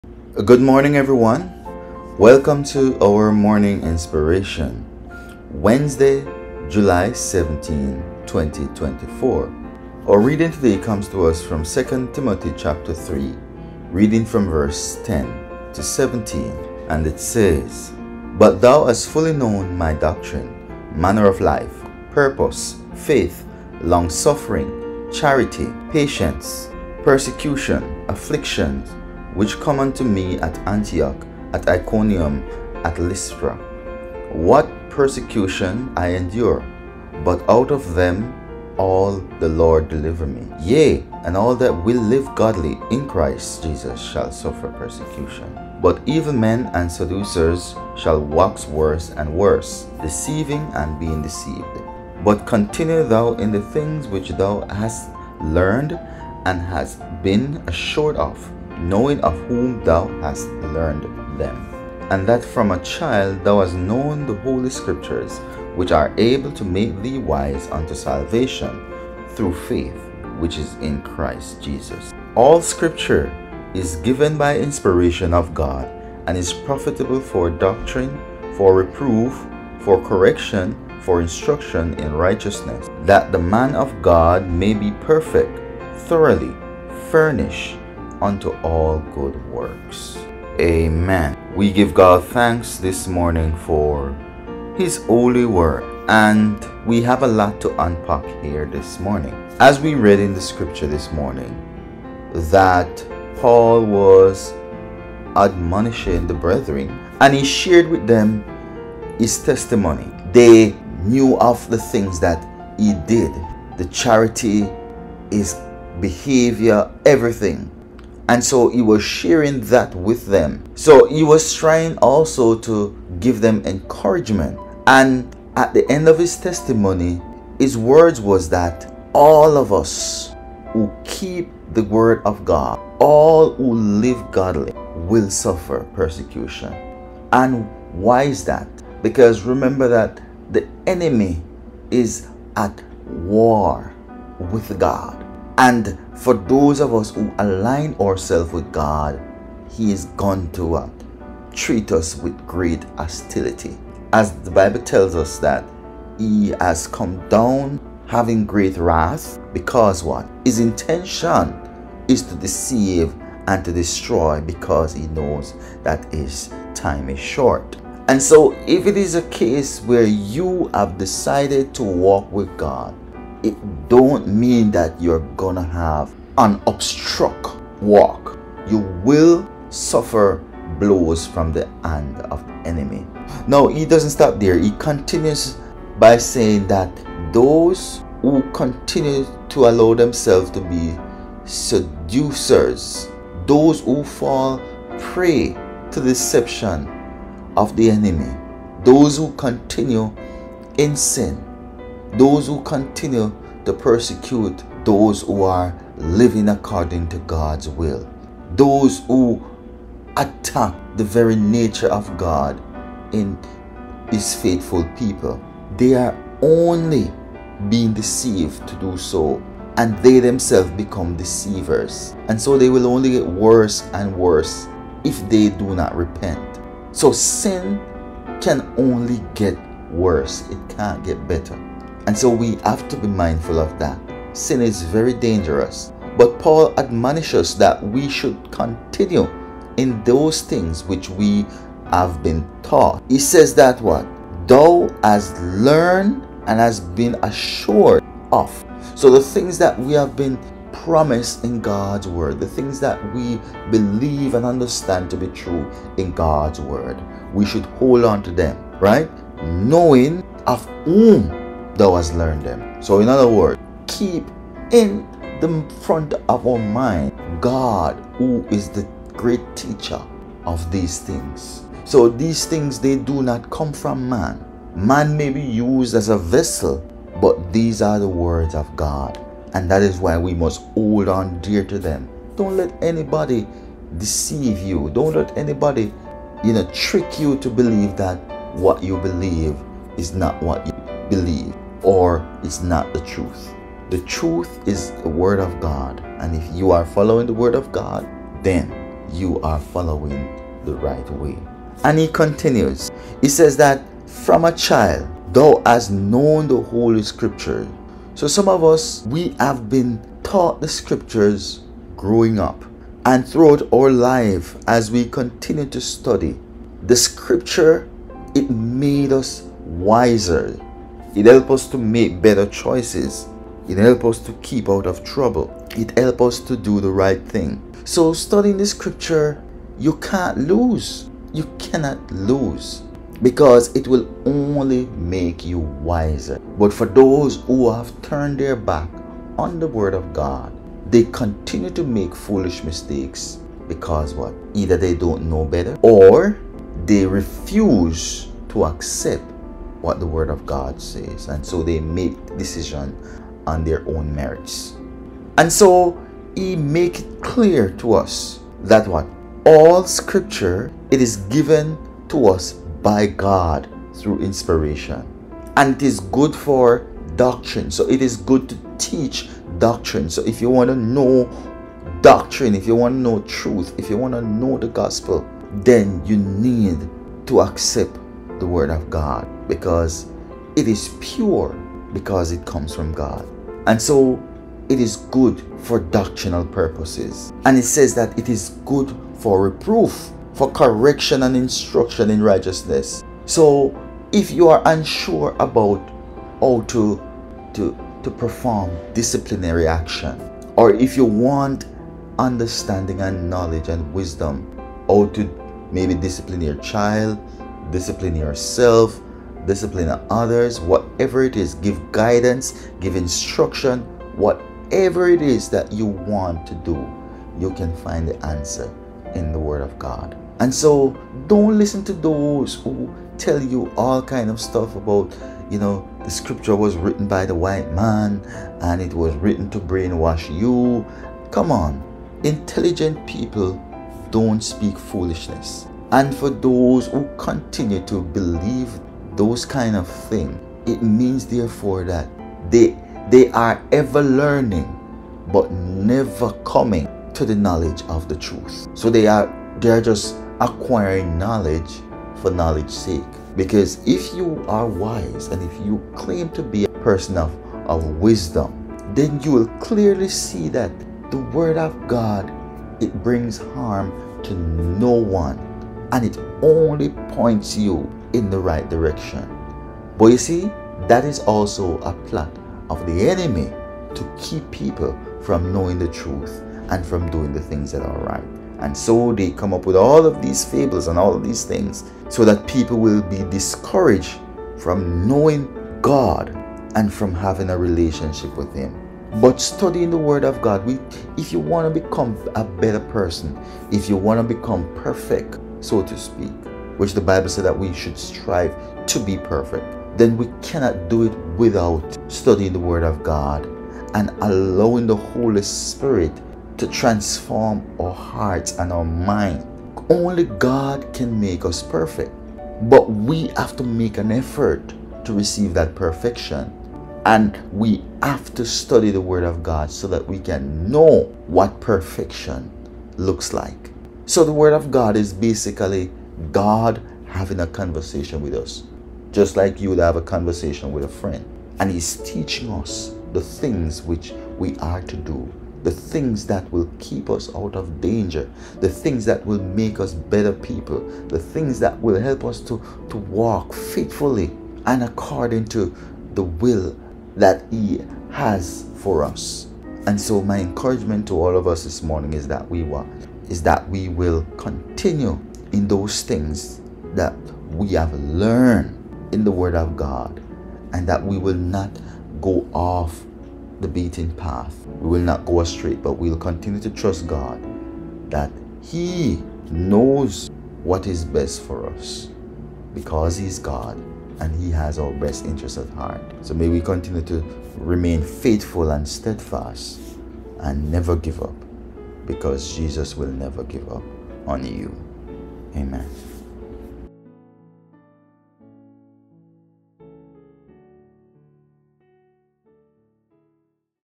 Morning Inspiration and Bible Verse. (2 Timothy 3:10-17) Wednesday, July 17, 2024
2 Timothy 3

10 But thou hast fully known my doctrine, manner of life, purpose, faith, longsuffering, charity, patience,

11 Persecutions, afflictions, which came unto me at Antioch, at Iconium, at Lystra; what persecutions I endured: but out of them all the Lord delivered me.

12 Yea, and all that will live godly in Christ Jesus shall suffer persecution.

13 But evil men and seducers shall wax worse and worse, deceiving, and being deceived.

14 But continue thou in the things which thou hast learned and hast been assured of, knowing of whom thou hast learned them;

15 And that from a child thou hast known the holy scriptures, which are able to make thee wise unto salvation through faith which is in Christ Jesus.

16 All scripture is given by inspiration of God, and is profitable for doctrine, for reproof, for correction, for instruction in righteousness:

17 That the man of God may be perfect, thoroughly furnished unto all good works.